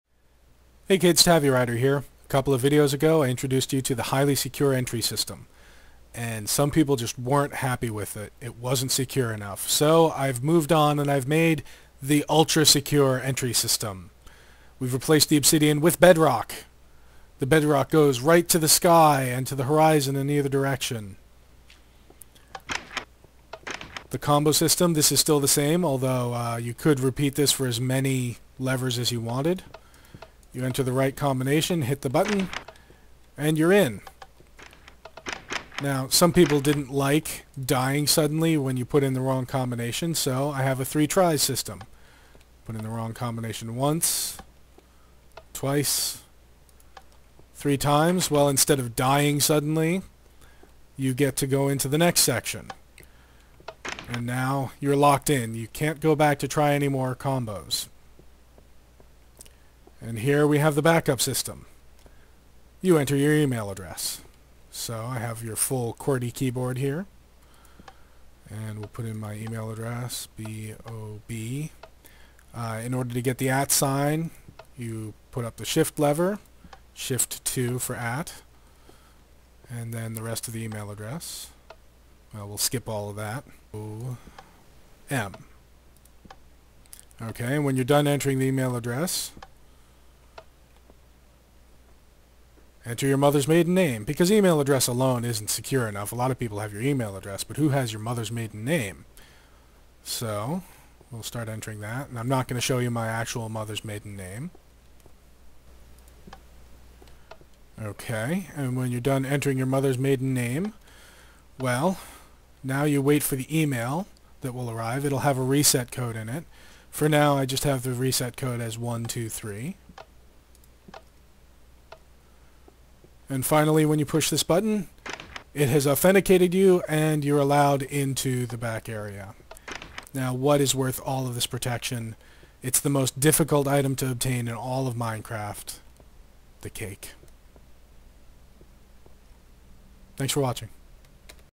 Ultra-Secure Entry System
My highly secure entry system wasn't enough for some people. So I've spent countless hours upgrading it to...

The Ultra-secure Entry System(TM)